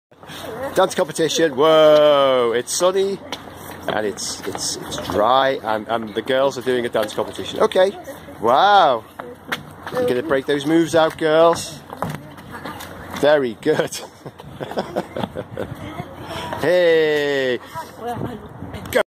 ZX Girls dance contest w Daddy :-) zx girls, dance girls
Impromptu moment caught today .
Zara n Xanthe Mc busting their moves

SM x